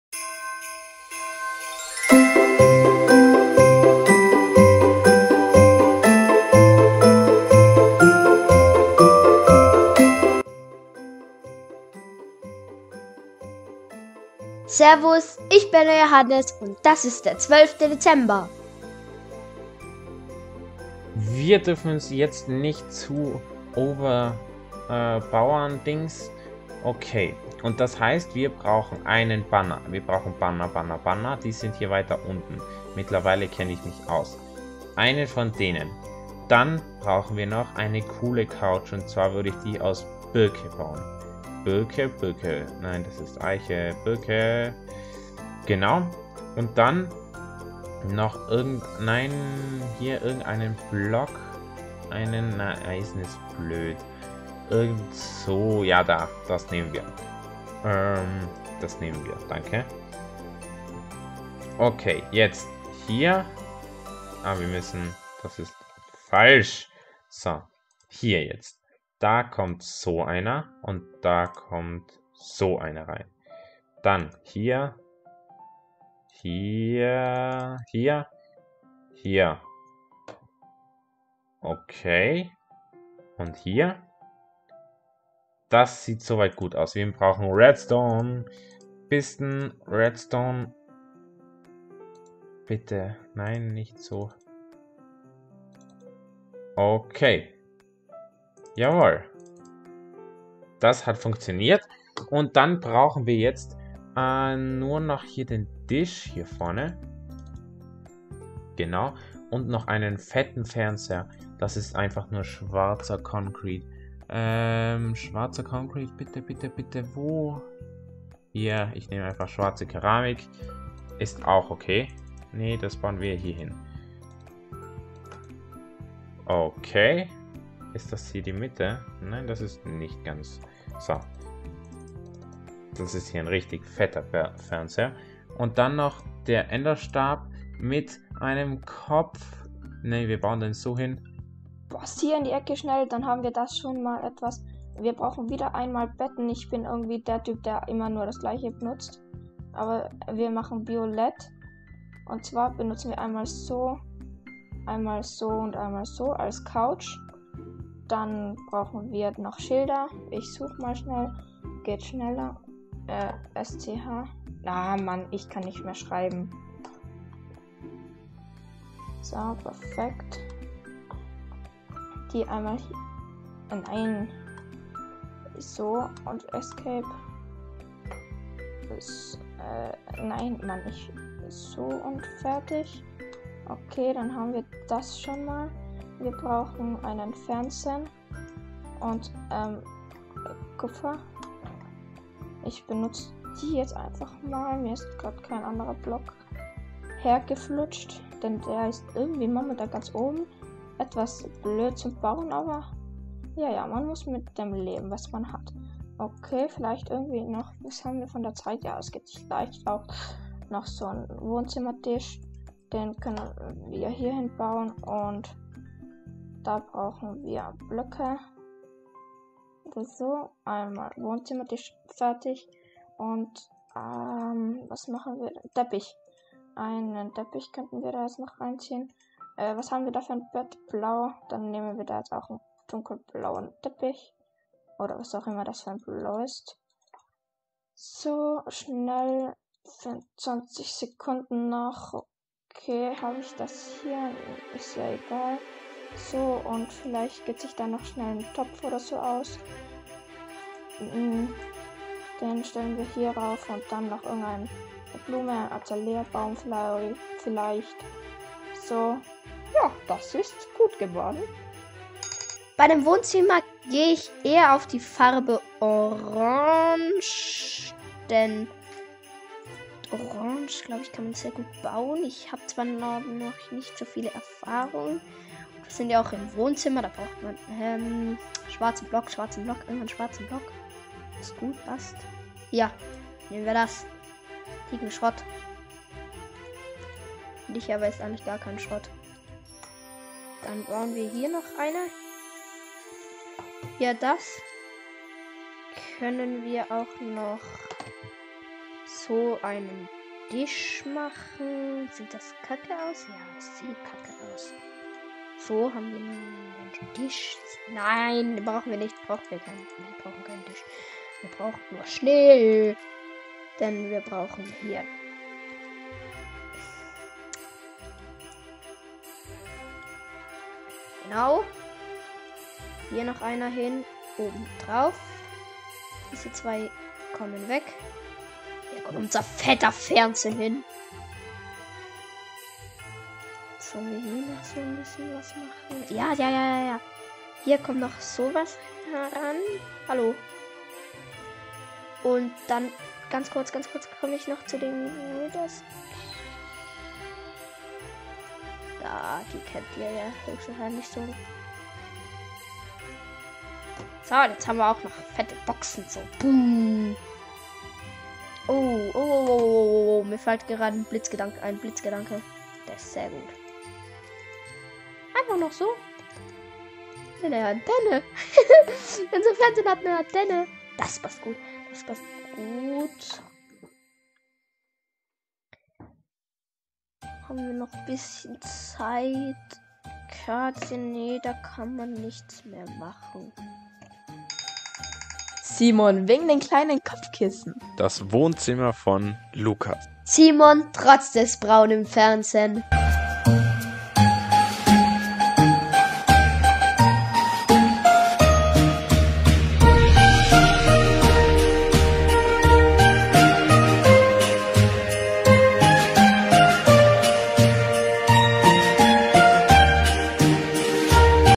Bitte Popcorn und Chips! - Klotzi LP Adventskalender
Das Wohnzimmer ist der Ort an dem man sich wohlfühlt und sich entspannen kann. In unseren Wohnzimmern darf ein Fernseher natürlich auch nicht fehlen, auch wenn sie aus etwas ungewöhnlichen Materialien Wie Kohle bestehen.